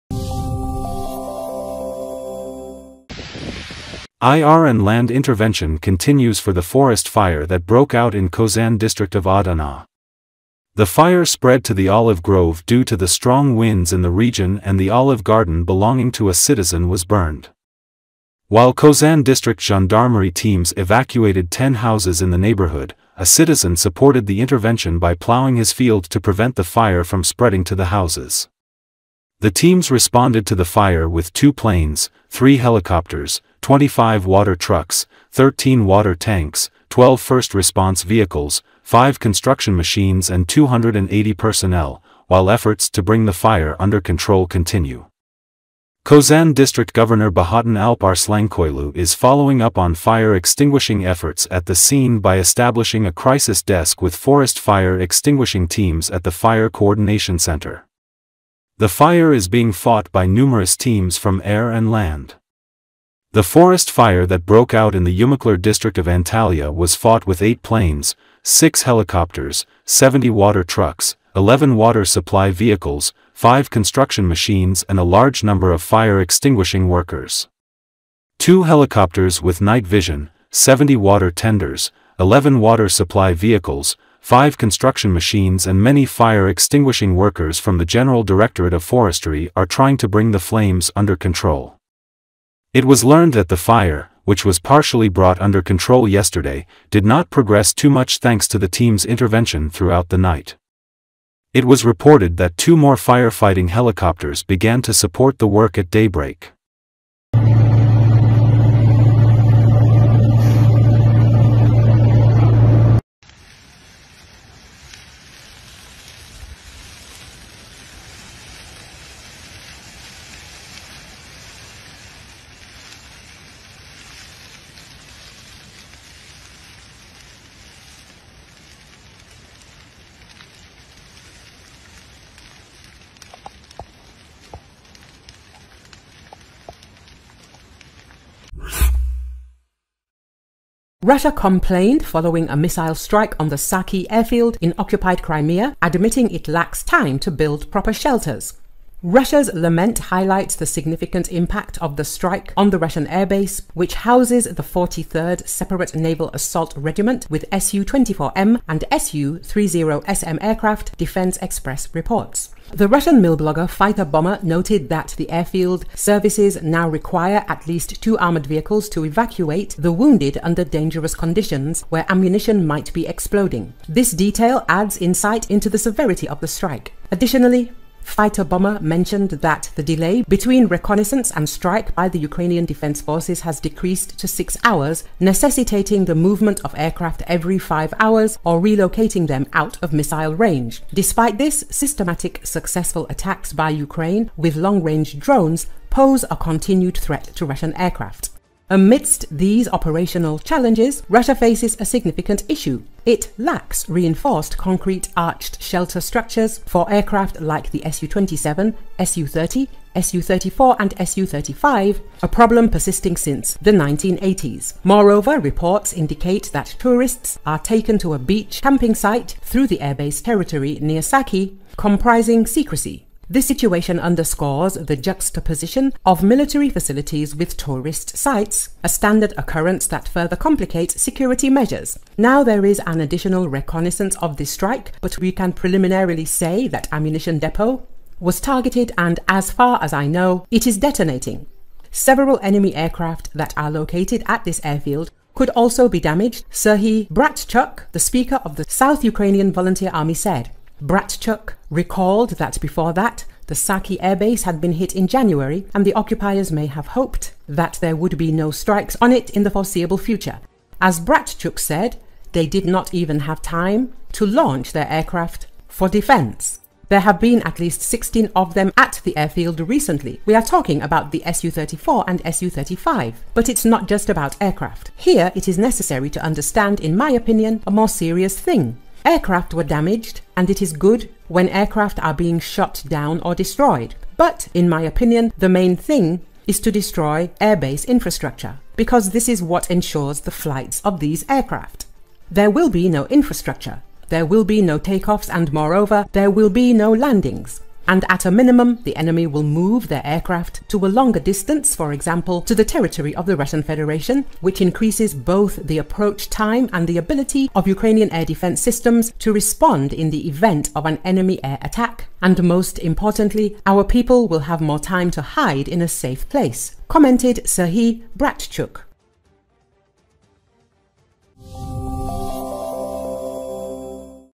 Strong forest fire in Turkiye's Adana - Evacuation underway
Air and land intervention continues for the forest fire that broke out in Kozan district of Adana. The fire spread to the olive grove due to the strong winds in the region and the olive garden belonging to a citizen was burned.

While Kozan District Gendarmerie teams evacuated 10 houses in the neighborhood, a citizen supported the intervention by plowing his field to prevent the fire from spreading to the houses. The teams responded to the fire with 2 planes, 3 helicopters, 25 water trucks, 13 water tanks, 12 first response vehicles, 5 construction machines and 280 personnel, while efforts to bring the fire under control continue.

Kozan District Governor Bahattin Alp Arslanköylü is following up on fire extinguishing efforts at the scene by establishing a crisis desk with forest fire extinguishing teams at the fire coordination center. The fire is being fought by numerous teams from air and land.

The forest fire that broke out in the Yumaklar District of Antalya was fought with 8 planes, 6 helicopters, 70 water trucks, 11 water supply vehicles, 5 construction machines and a large number of fire extinguishing workers. Two helicopters with night vision, 70 water tenders, 11 water supply vehicles, 5 construction machines and many fire extinguishing workers from the General Directorate of Forestry are trying to bring the flames under control.

It was learned that the fire, which was partially brought under control yesterday, did not progress too much thanks to the teams' intervention throughout the night.